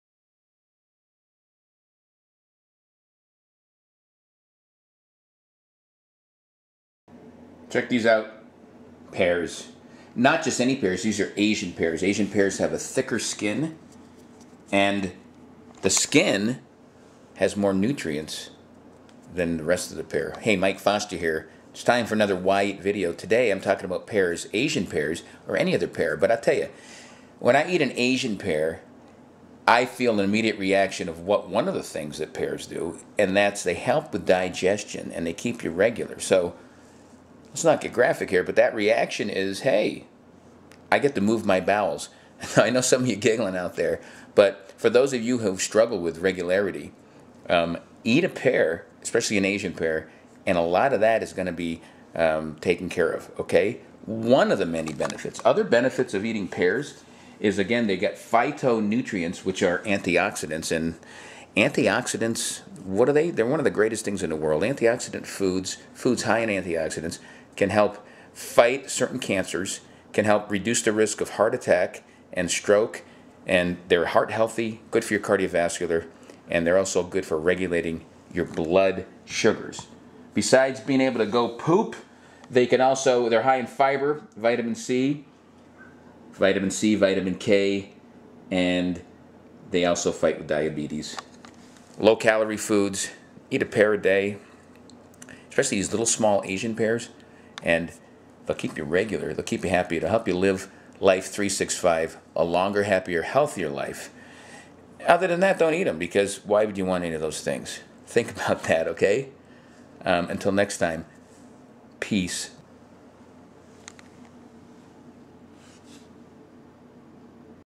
Why Eat Asian Pears?
Delicious and loaded with vitamins, asian pears can also help aid in digestion, fight cancer and lower blood sugars.  High in potassium, fiber, vitamin K, C, and copper, these babies can also fight obesity and lower  your blood pressure.